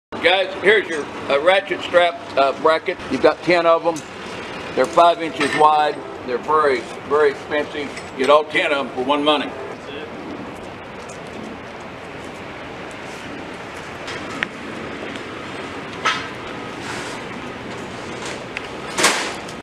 CUSTOM MADE RATCHET STRAP BRACKETS QTY10 For Sale
Custom Made Ratchet Strap Brackets Qty10 -5inch Wide, *See Pics & Video For More Details *Sells Absolute! ~Please Read Terms & Conditions Before Bidding*